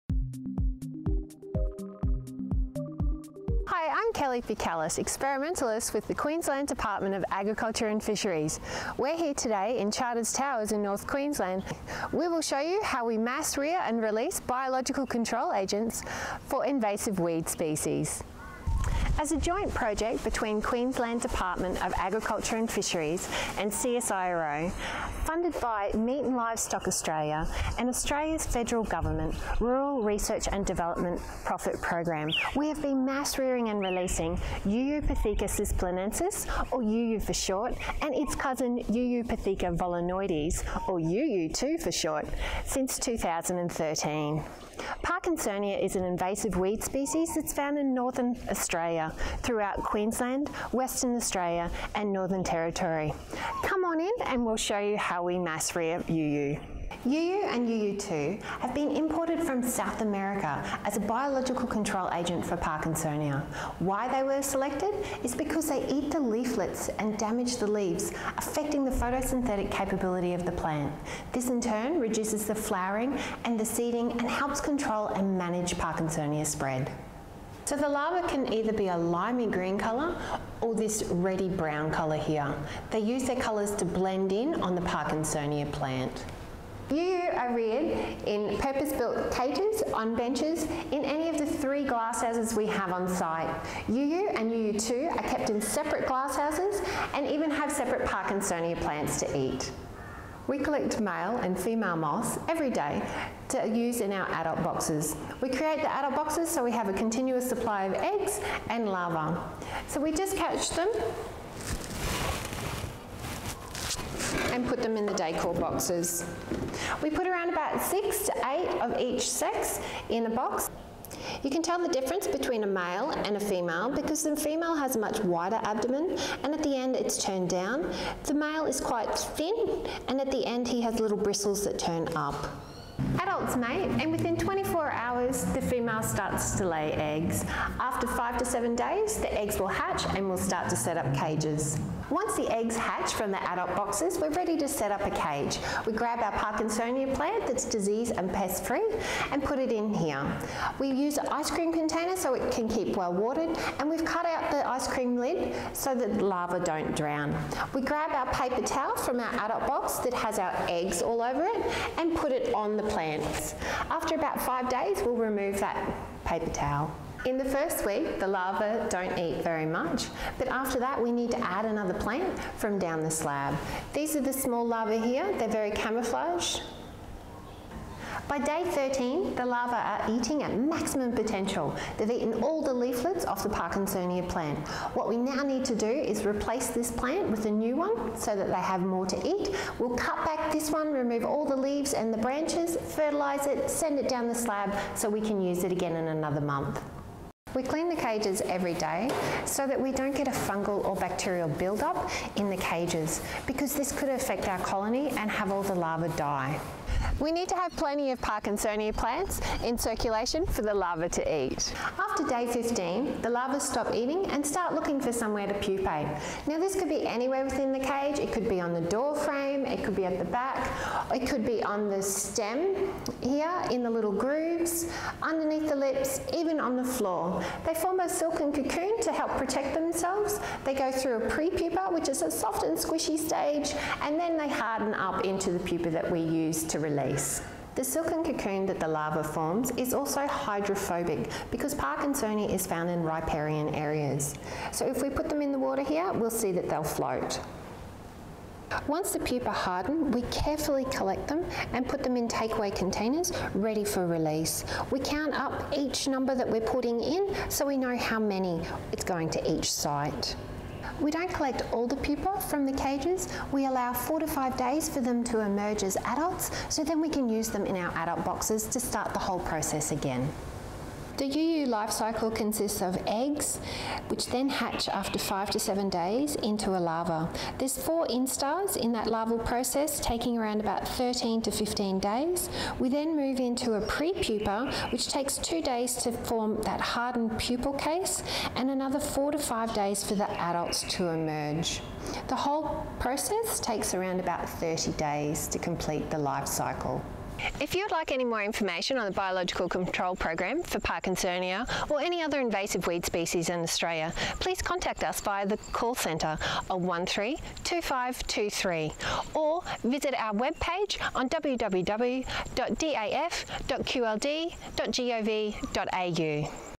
Mass-rearing of the biological control agents UU & UU2
Methodology of mass-rearing and biology of the biological control agents UU (Eueupithecia cisplatensis and E. vollonoides) on Parkinsonia (Parkinsonia aculeata).

A showcase of the mass-rearing and biology of the introduced biological control agent Eueupithecia cisplatensis (UU) and Eueupithecia vollonoides (UU2), which were introduced to assist with the control of invasive weed species Parkinsonia aculeata.

Funding partnerships with MLA, CSIRO and Australia's Federal Department of Agriculture and Water Resources, Queensland’s Department of Agriculture and Fisheries mass-reared the agents at Tropical Weeds Research Centre in Charters Towers. This project involved partnerships with many regional landholders, stakeholders, and local and state government agencies throughout Queensland, Northern Territory and Western Australia.